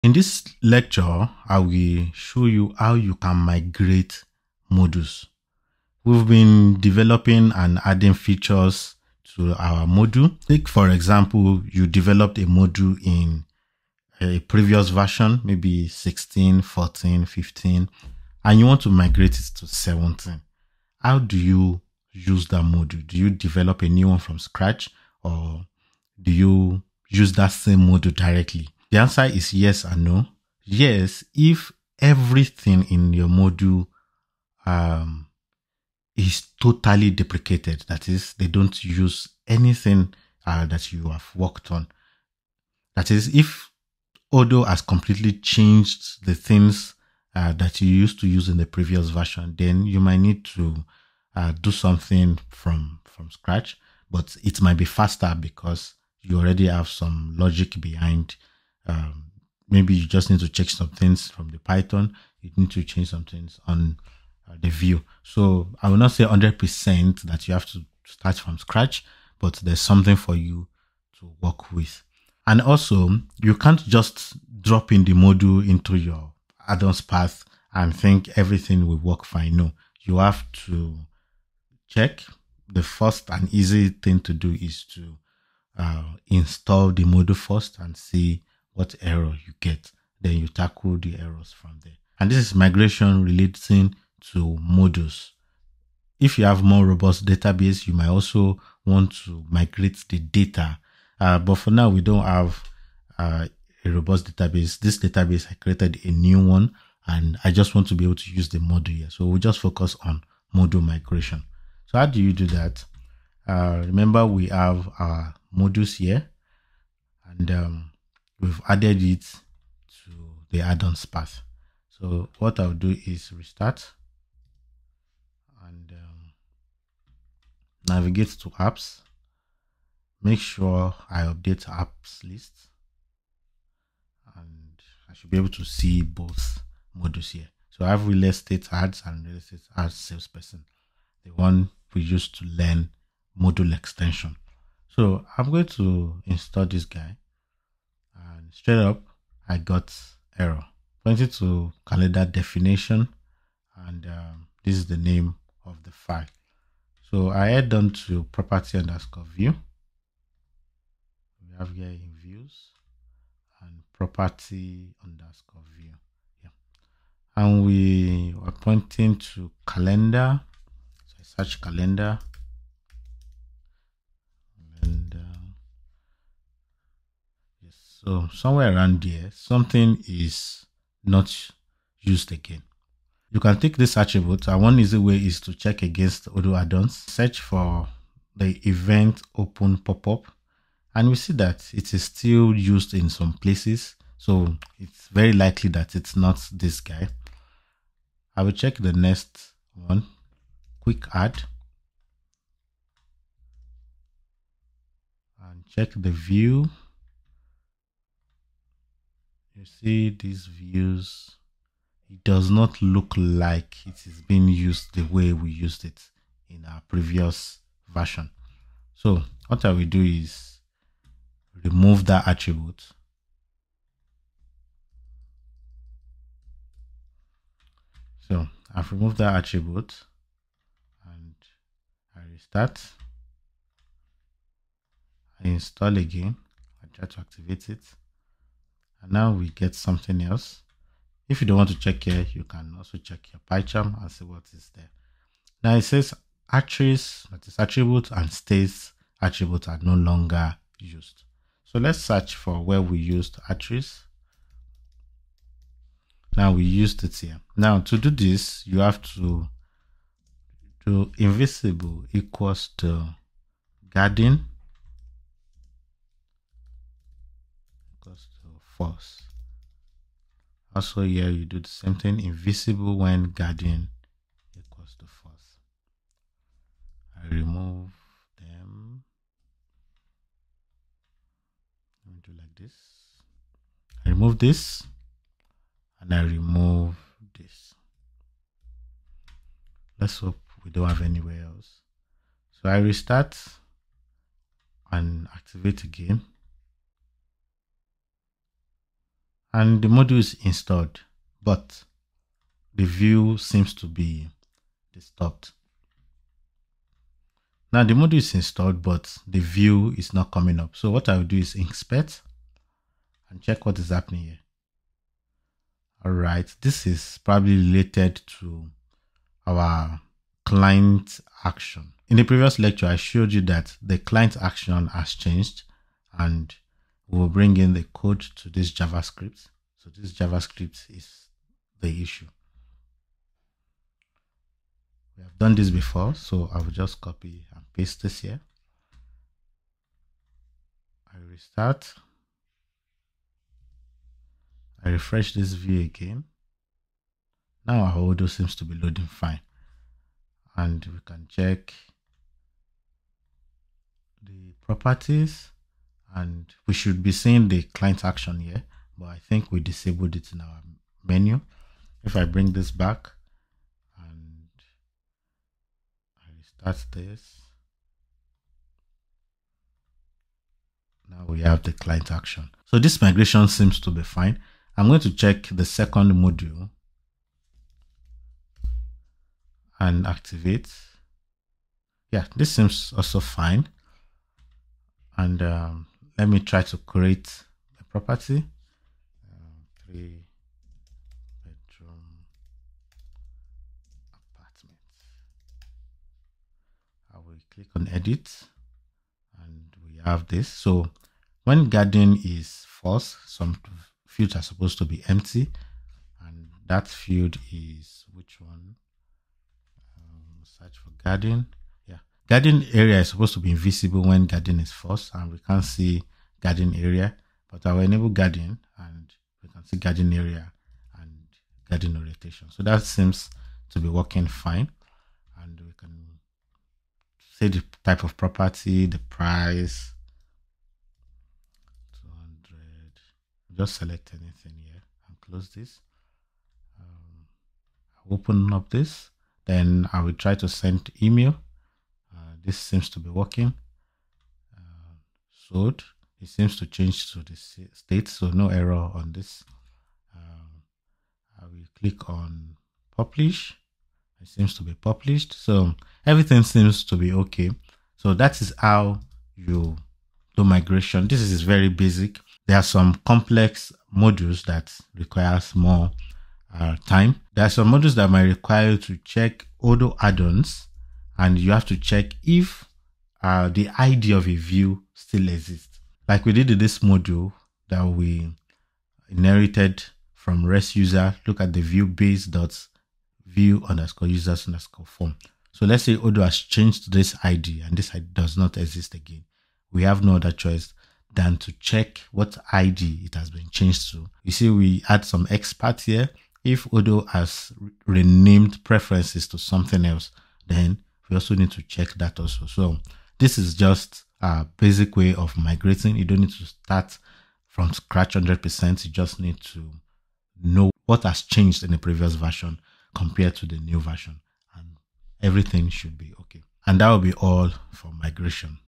Migrating Odoo Modules from 16.0 to 17.0
In this cast, I will show you how you can migrate odoo modules from version 16.0 to 17.0.

Follow along to ensure your migration is smooth and efficient, allowing you to take full advantage of the new features and improvements in Odoo 17.0

If you want to learn more about developing modules in odoo, check out my course - The Complete Odoo Technical Course 2023: From Zero to Hero"